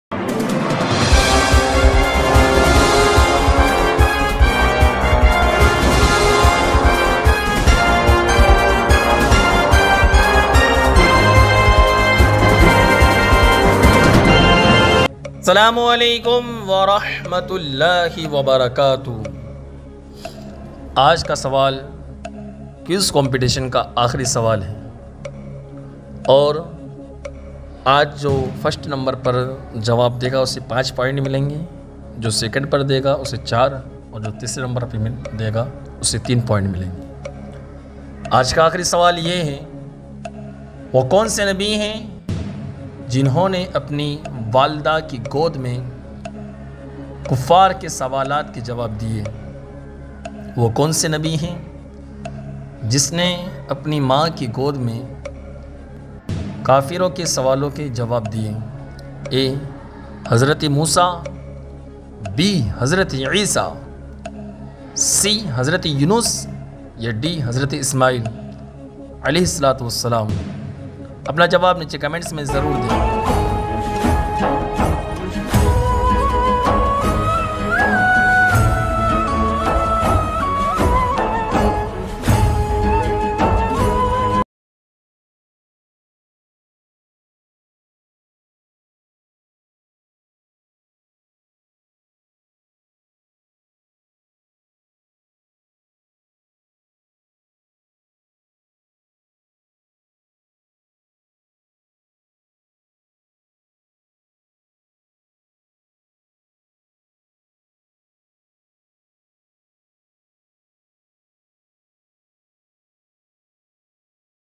वह कौन से नबी है जिन्होंने मां की गोद में काफिरों के सवाल का जवाब
Who is the prophet who answered the question of infidels in mother's lap